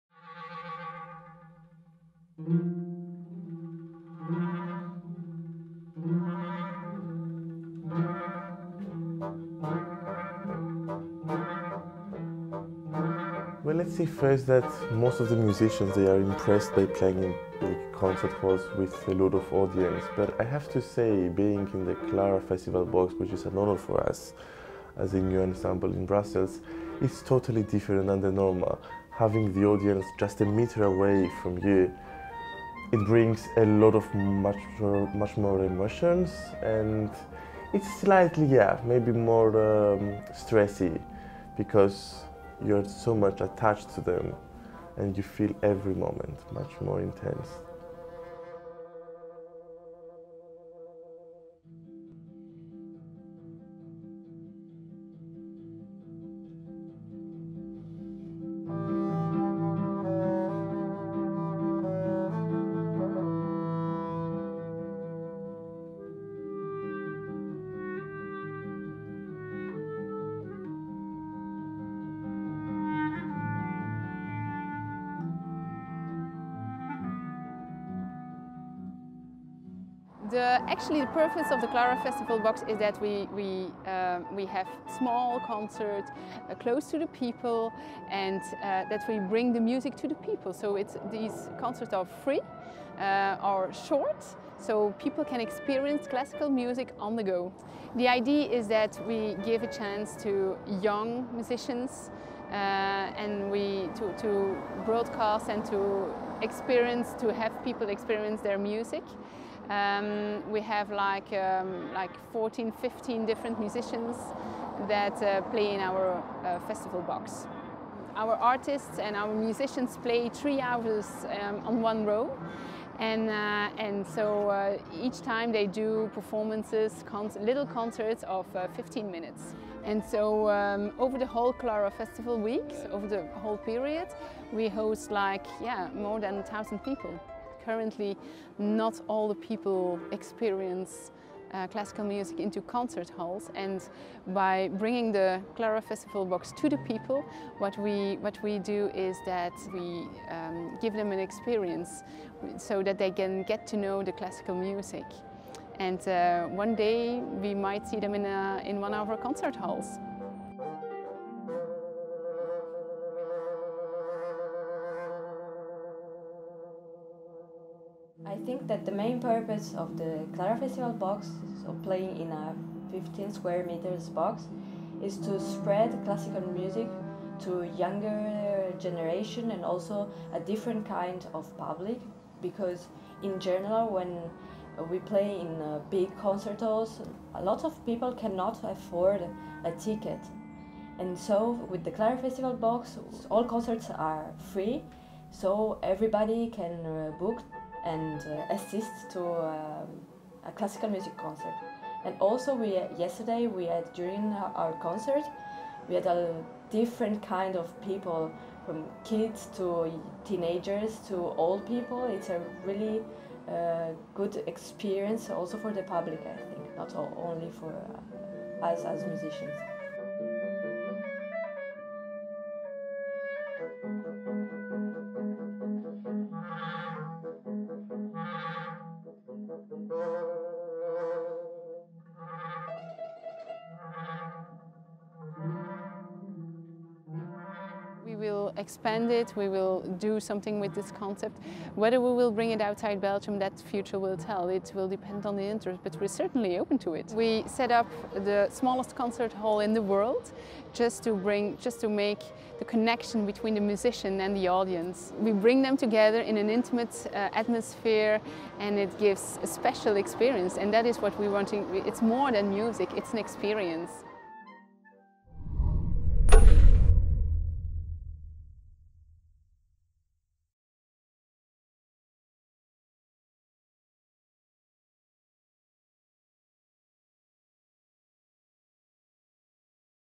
Inside The World's Smallest Concert Hall
Barely 15 sq ft in size and made entirely out of glass, the Klarafestival's Box is the smallest concert venue in the world. Based in Brussels, the venue can hold between three and six audience members depending on the performance.